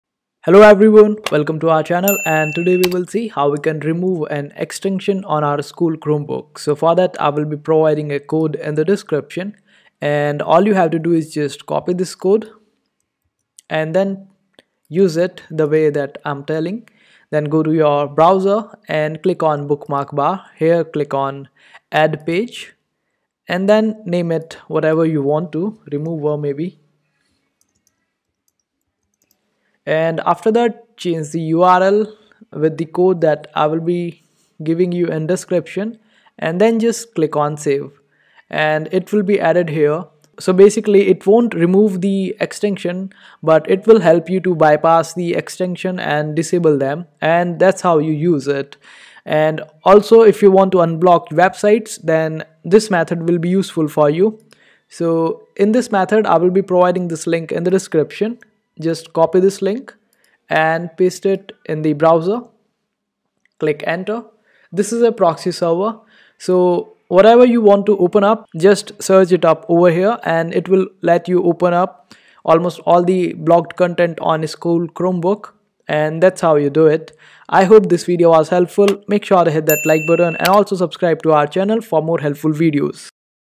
How To Remove Extension On School Chromebook (EASY WAY!)
How To Remove Extension On School Chromebook. It’s a very easy tutorial, I will explain everything to you step by step.